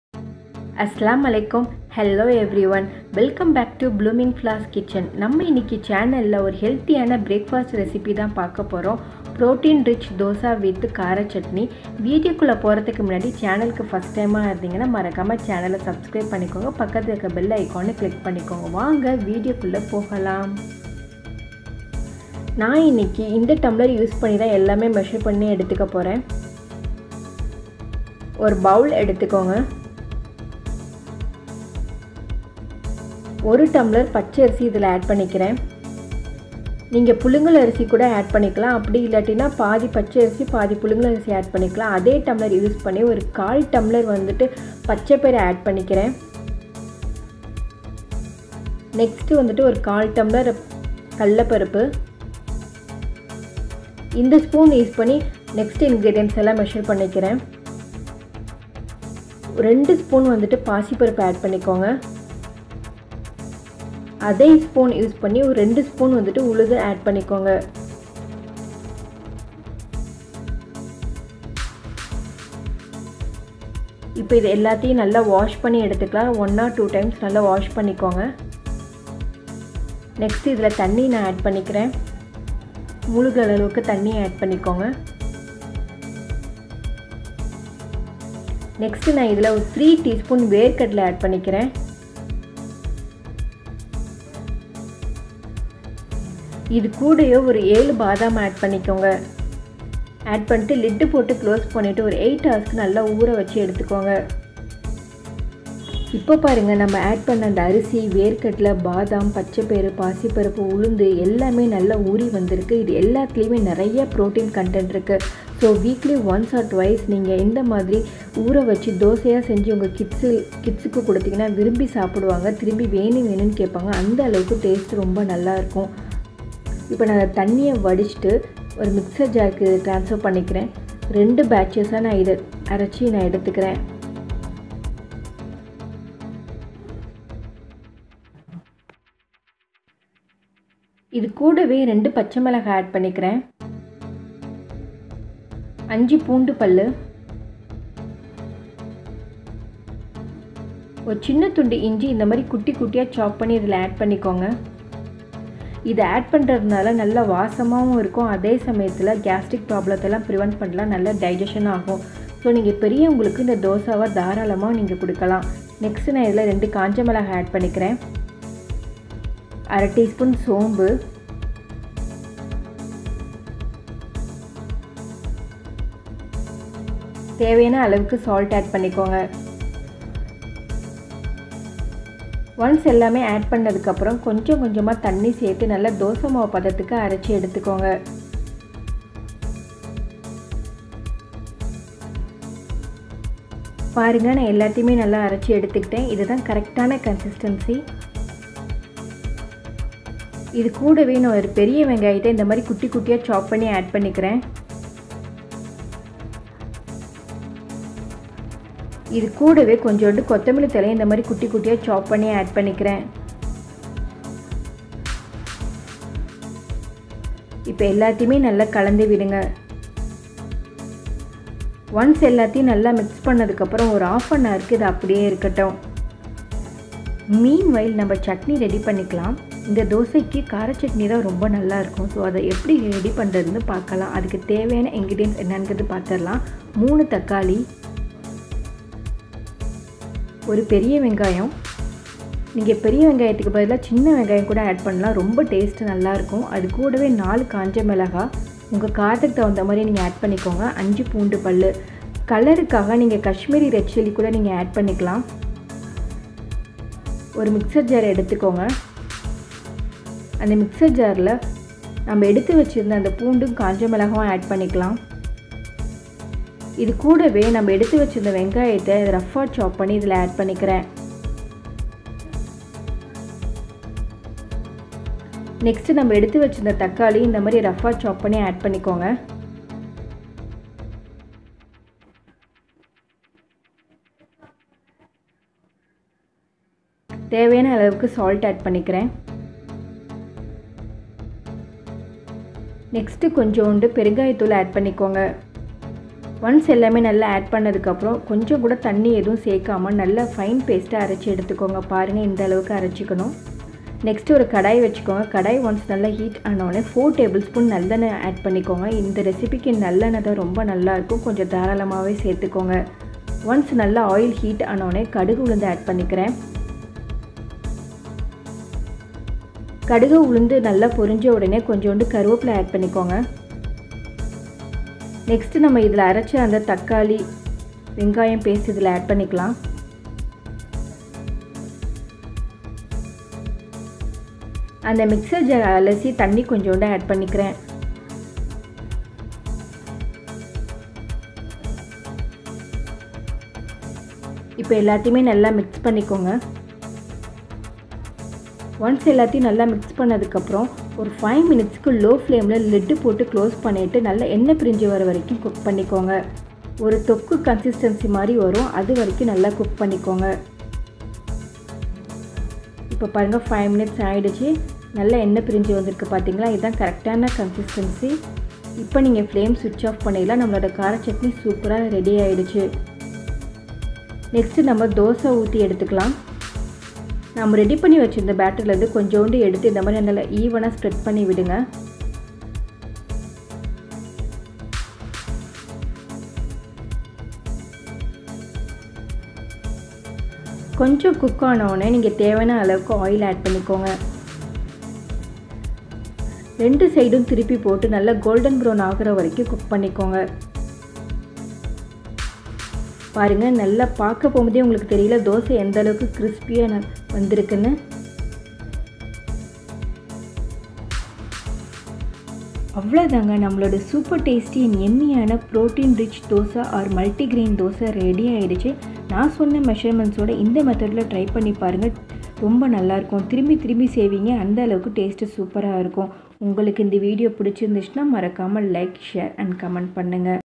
Healthy breakfast recipe | protein rich Dosa |Kaara chutney | Multigrain Dosa
Hello friends
                  In this video I share healthy breakfast recipe Multigrain Dosa or protein rich Dosa which is very easy to make and tastes Gud. This protein rich Dosa is Gud for health. This protein rich Dosa includes Multigrain like, Moong dal, Urad dal, peanut, badam, rice, Bengal gram, pacha peyaru All are rich in protein so try to take it in ur once or twice in a week. Definitely kids will love this recipe.

Ingredients
Rice-1glass
Pacha payaru-1/4glass
Bengal gram -1/4glass
Moong dal -2spoon
Urad dal -2spoon
Peanut -3tsp
Badam -7no
Salt to taste
Onion -1no
Red chilli -2no
Green chillies -2no
Fennel-1/2tsp
Coriander leaves chopped -few
Garlic pods-5nos

Kaara chutney (ingredients)
Onion-1no
Tomato -3nos
Red chilli -4nos
Hing -1/2tsp
Salt to taste
Garlic pods-5nos